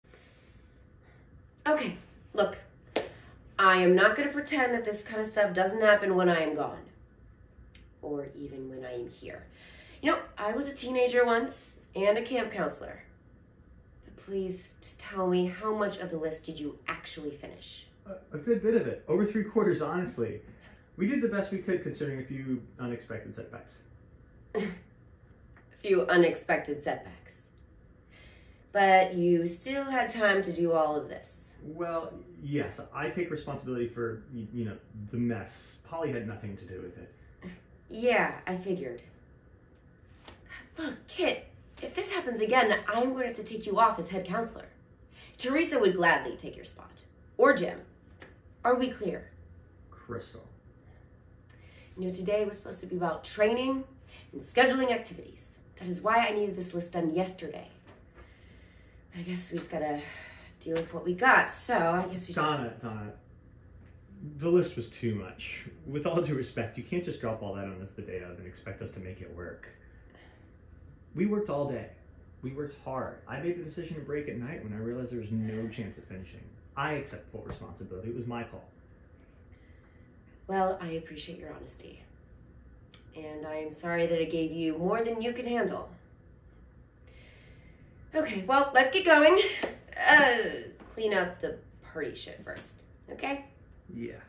Camp Counselor
A head camp counselor must reprimand a group of newbie counselors after discovering that instead of doing their jobs, they had a party